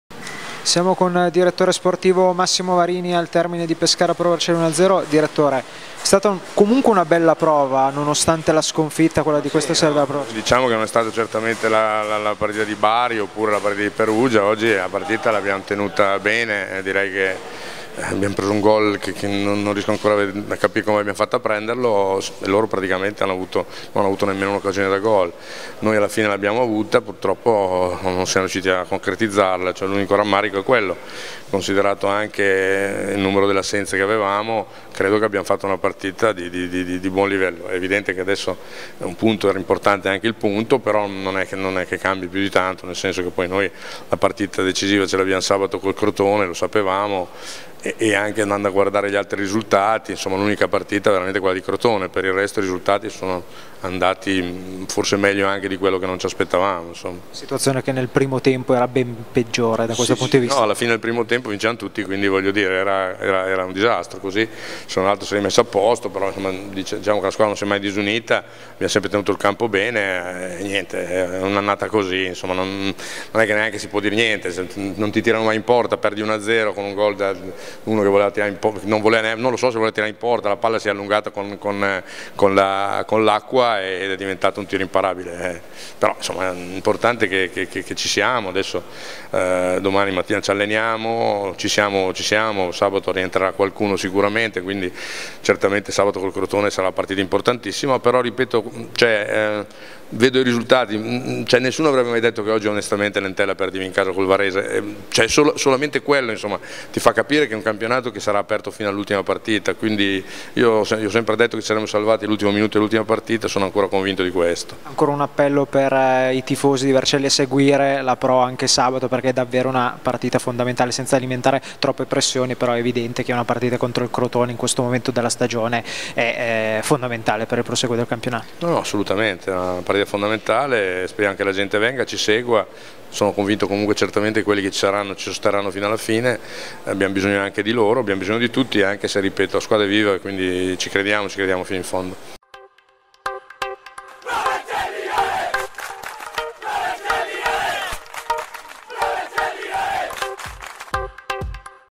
PESCARA-PRO VERCELLI: l'intervista al ds Massimo Varini
La video intervista al direttore sportivo della Pro Vercelli, Massimo Varini, al termine di Pescara-Pro Vercelli del 28/04/2015